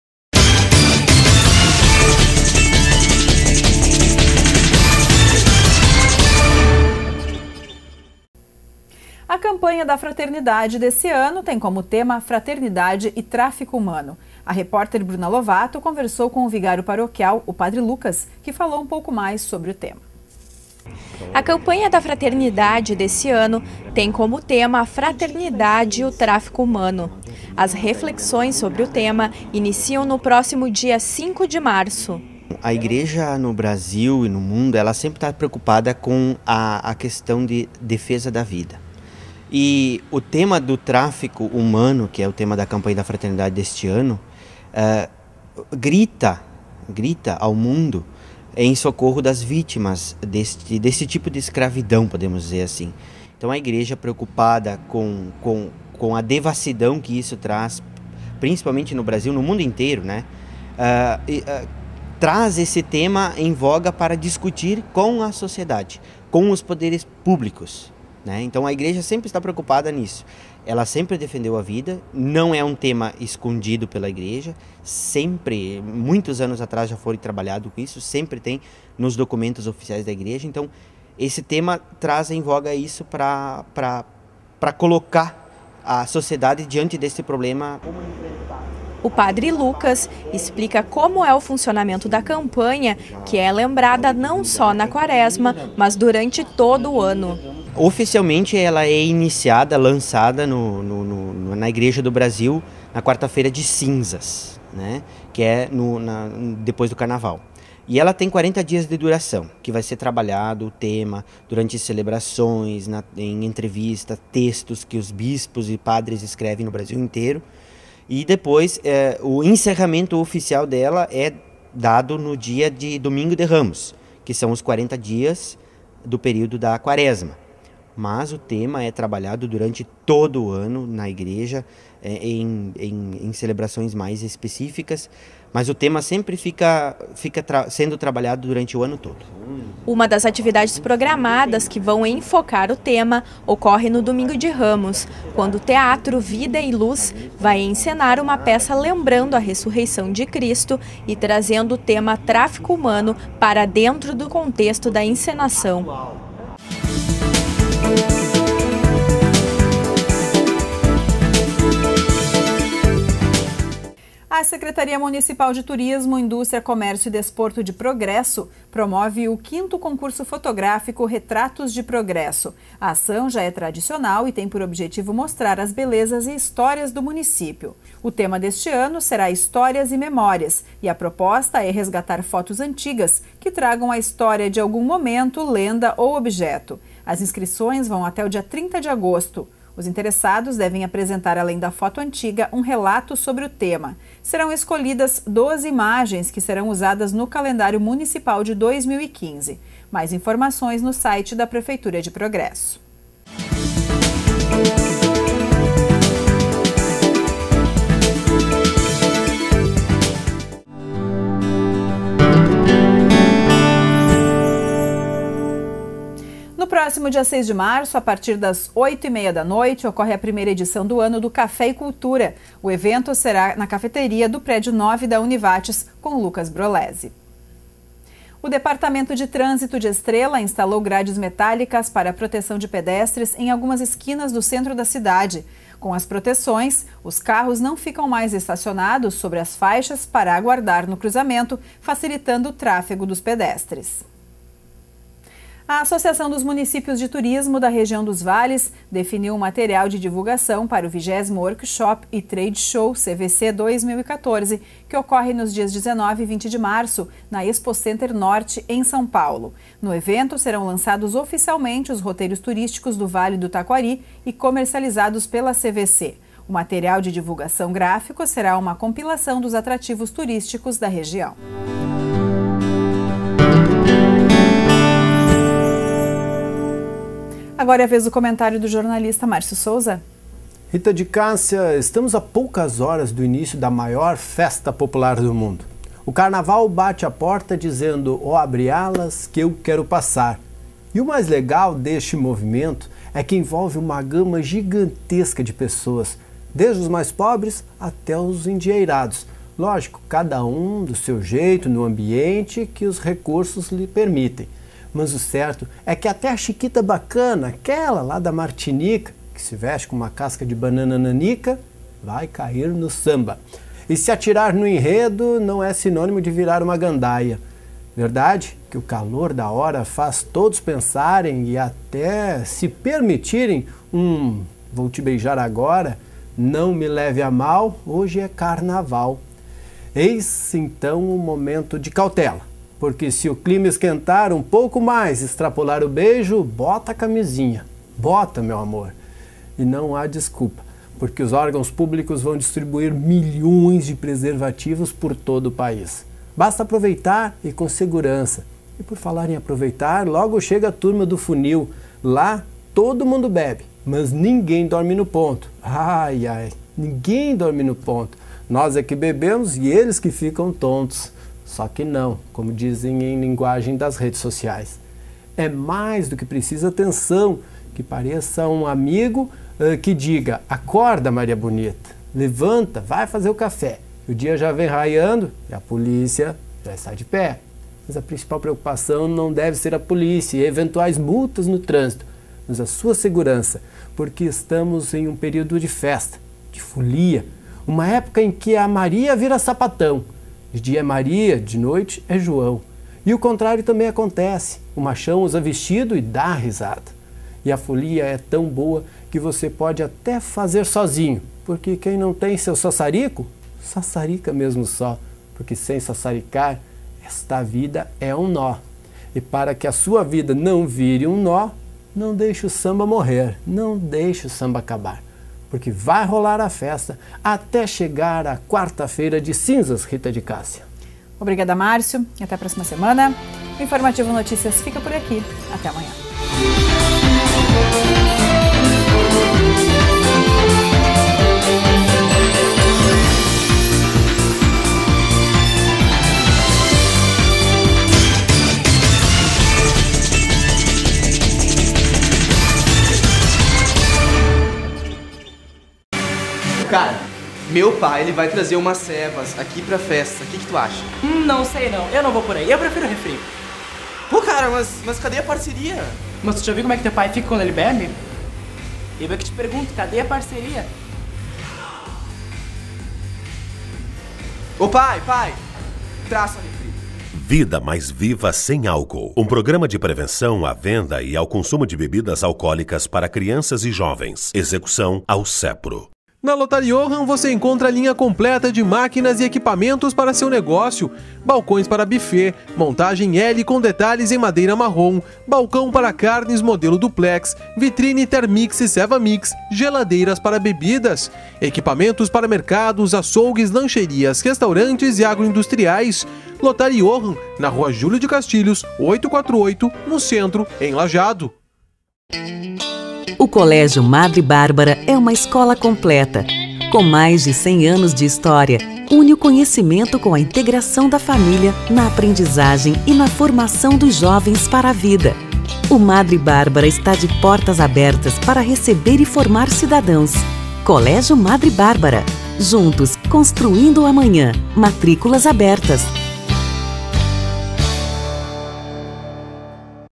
Informativo Notícias 26 02 2014 parte 2
No Informativo Notícias desta quarta-feira, 26 de fevereiro de 2014, com Rita de Cássia, confira a visita da comitiva da 6ª ExpoRoca e 9ª Fecarpa; C campanha da fraternidade 2014 tem como tema fraternidade e tráfico humano; Apasfa precisa de funcionário; Pelo Vale; Oportunidades; TV Informativo Comunidade e os comentários  do professor João Timotheo e do jornalista Márcio Souza.